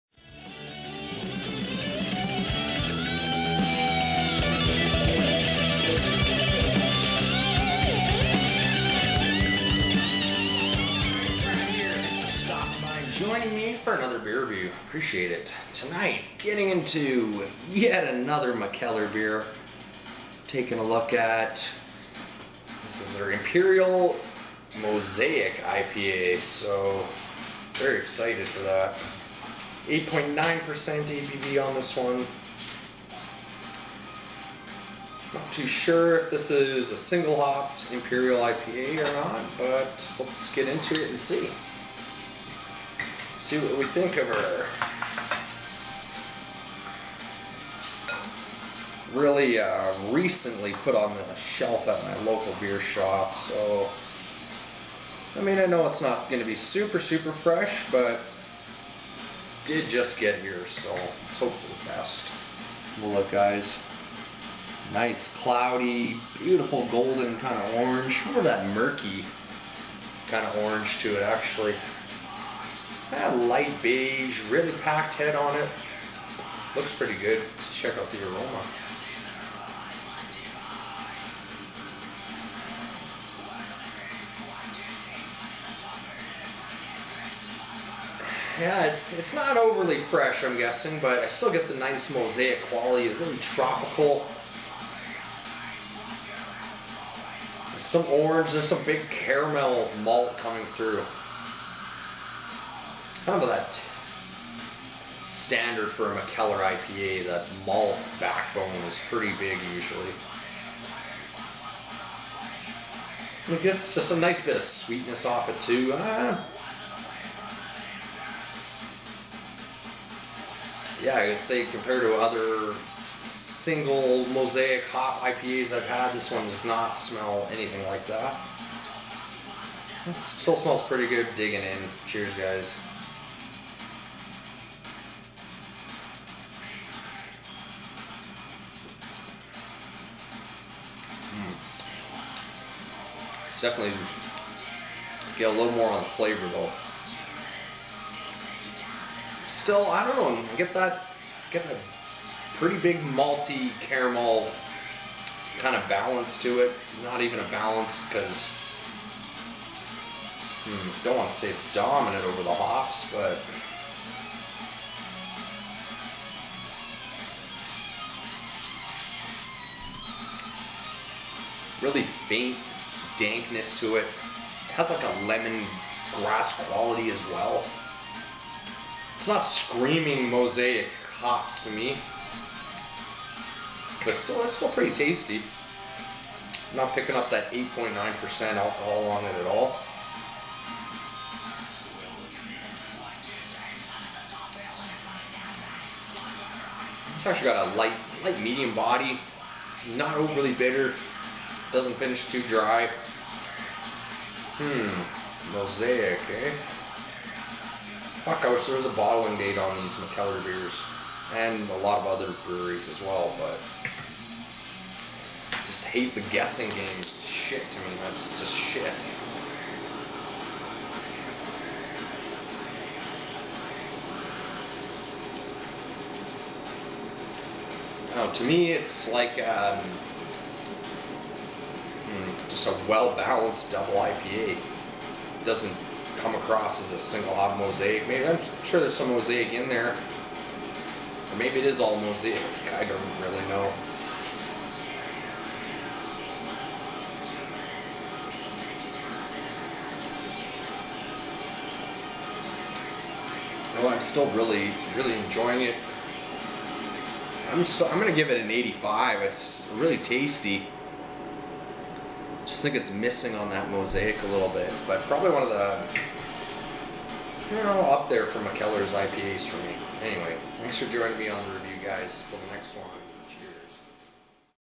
Beer Review: Mikkeller Mosaic DIPA (Review #426)
Yet another Mikkeller brew on deck lets get at it Cheers!!